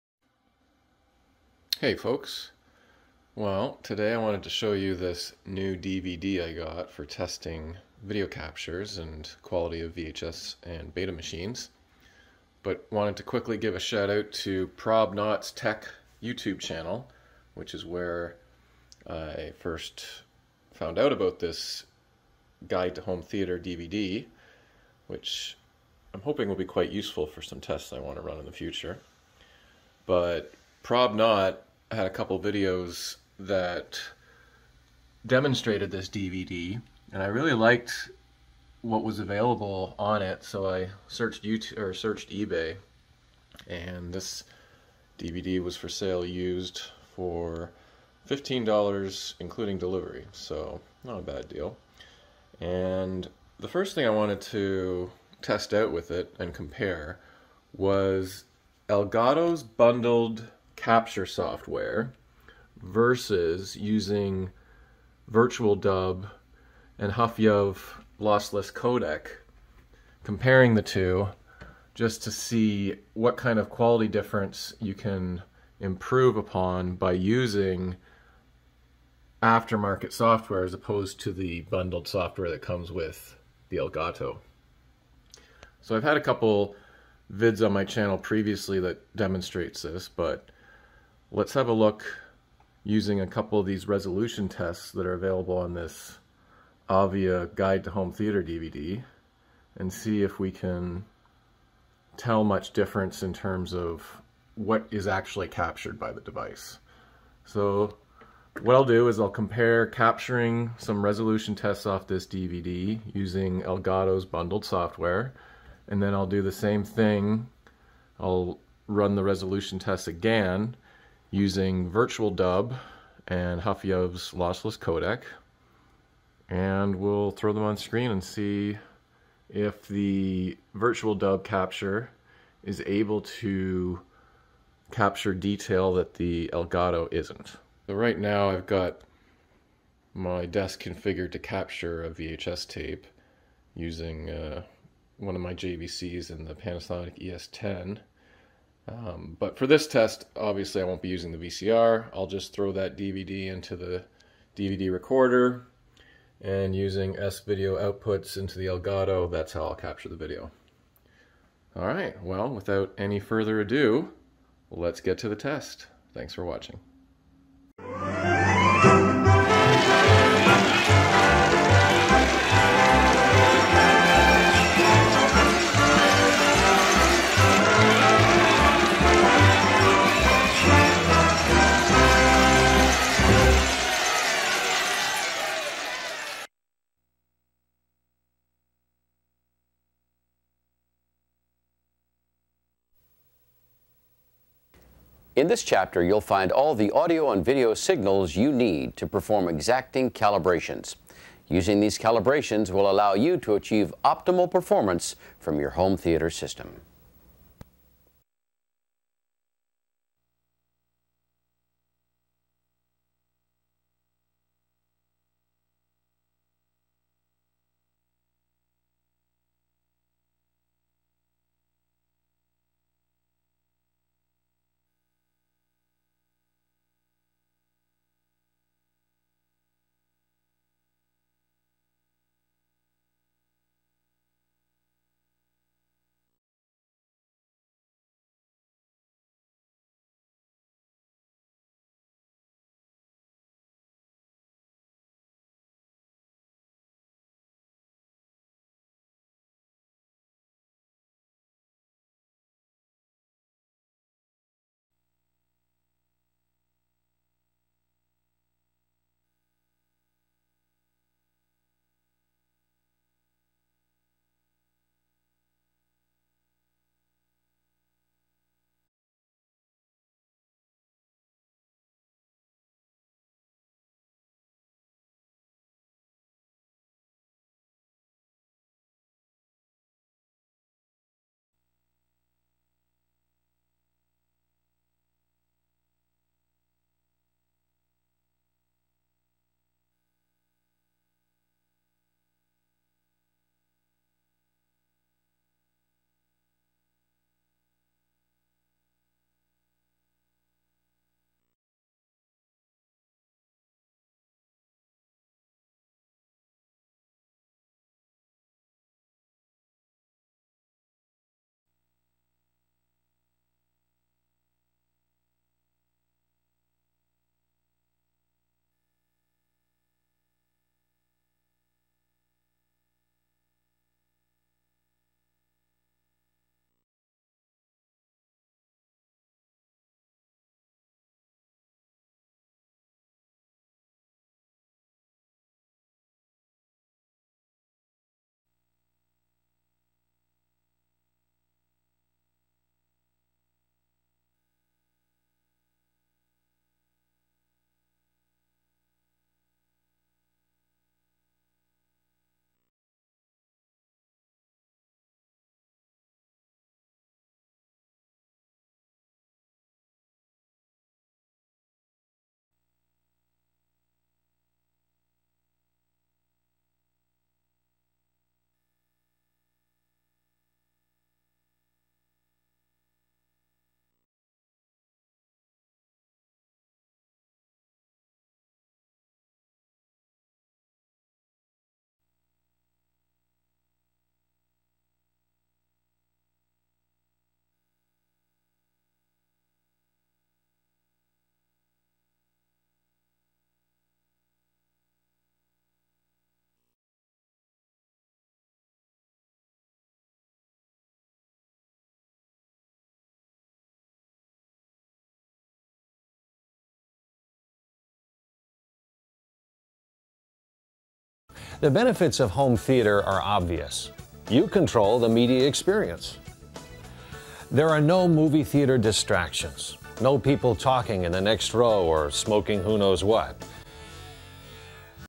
Elgato's Bundled Software vs VirtualDub and huffyuv Lossless Codec
Comparing resolution and sharpness captures between Elgato's bundled software and VirtualDub / HUFFYUV lossless codec method.

Shout-out to ProbNot ) for introducing me to this DVD through his YouTube channel.  Check him out!

For this initial test, default proc-amp settings were used in Virtualdub, which is why the captured footage is over-exposed.